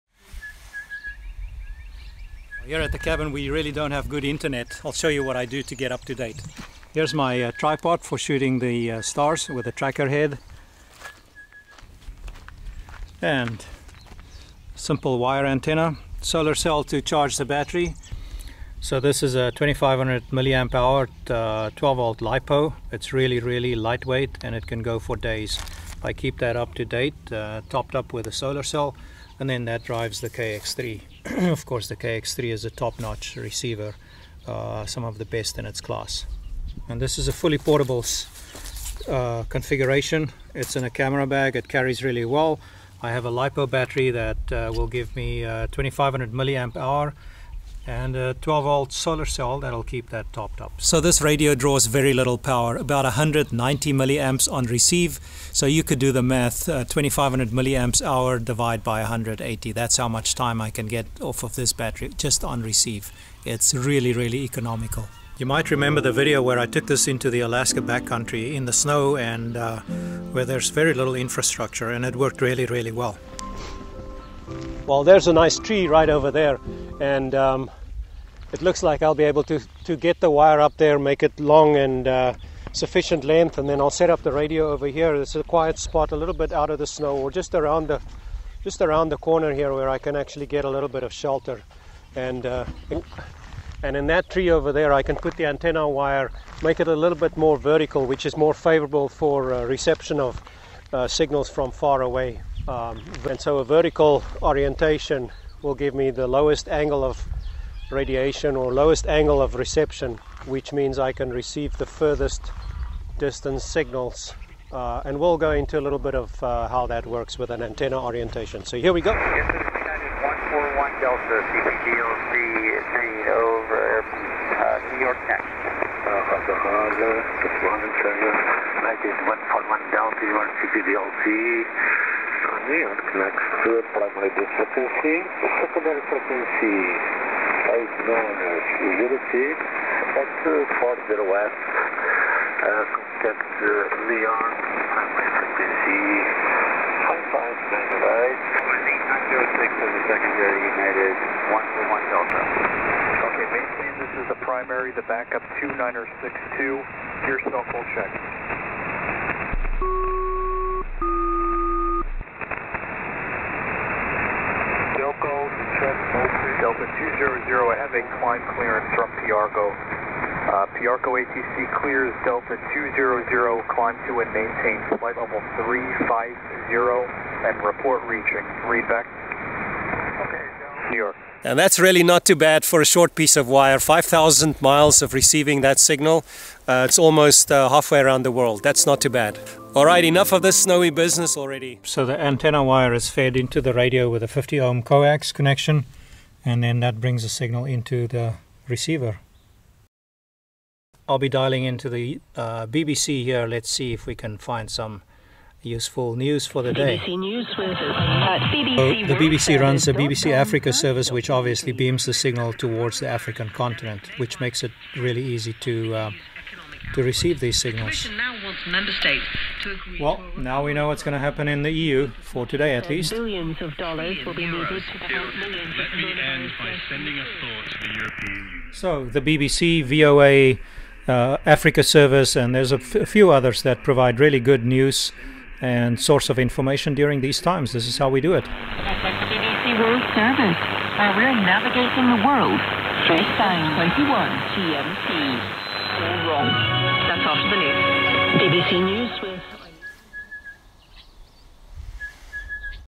HF Radio - Solar power Emergency communications - how I get World News without the internet.
With Emergency communications, using a portable solar powered HF Radio, I show how to get the news without the use of the internet.  In remote areas,  no cellphone or internet coverage, we stay updated with world news, by tuning into the Shortwave news broadcasts.

In this video, I show you my emergency communications, portable radio station setup, I use for effectively staying in contact with the world.